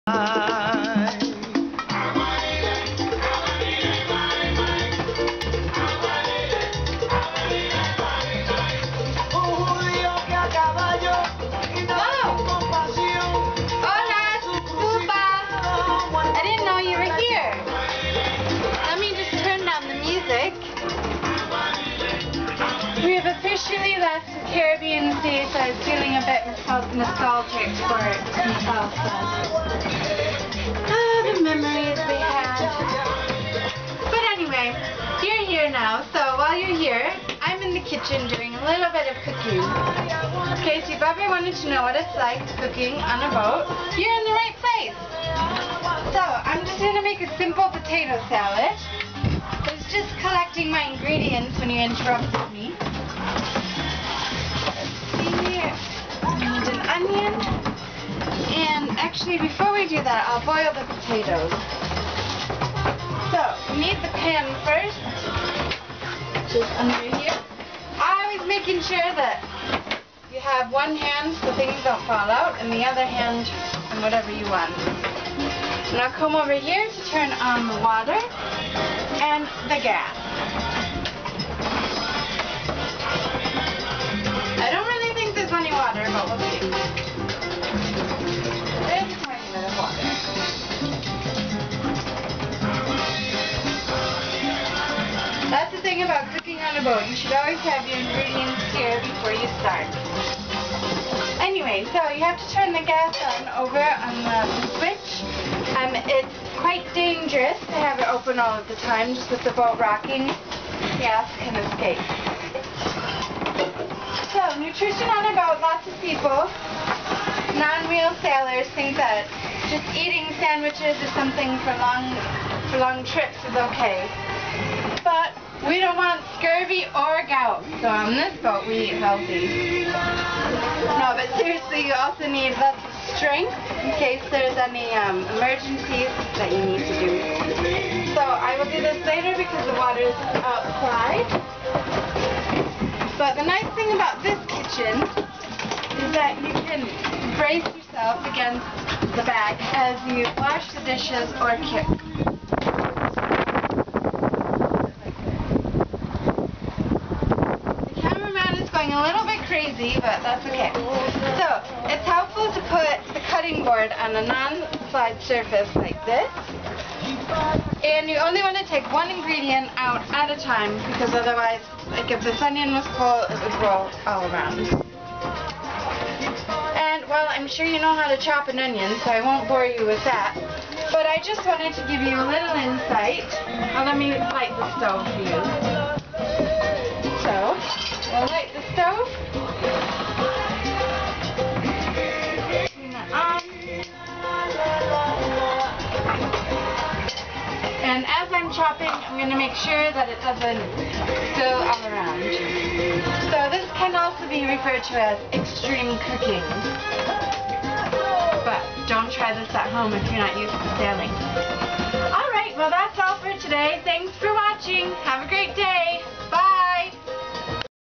Cooking in a boat on the open sea
Ever wondered how us seasoned sailors cook when we're offshore? You've come to the right place!